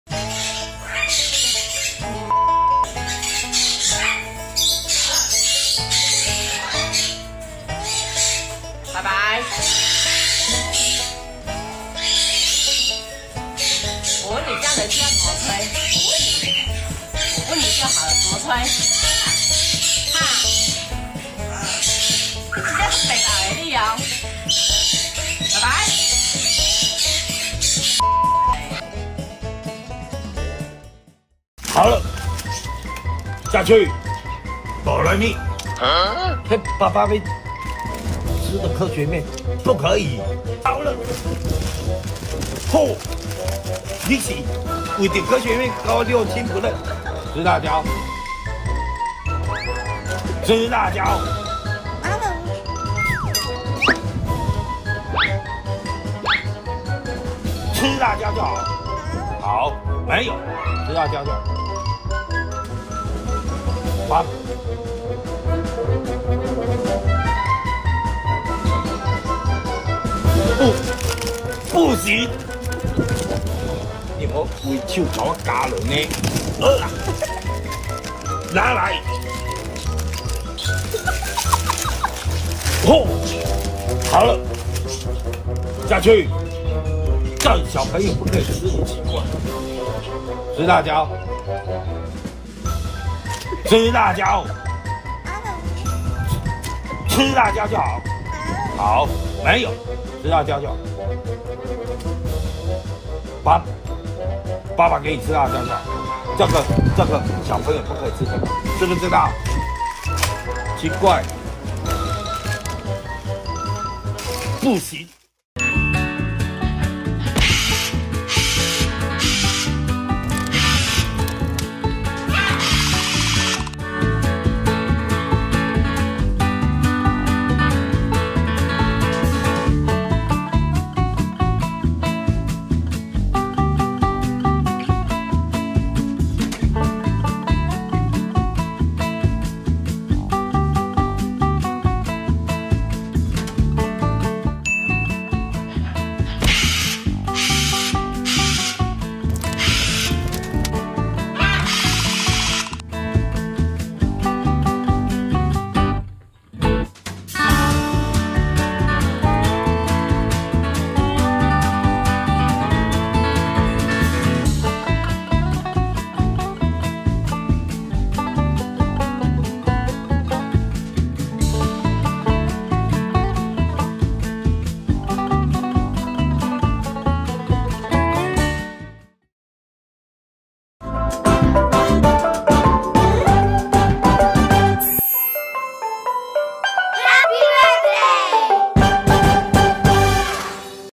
鸚鵡分屍冷氣遙控器零件扔滿地 站門上拒認罪還驕傲看闖禍成果│寵物│鳥│Pet│
影片授權：網友 林姿余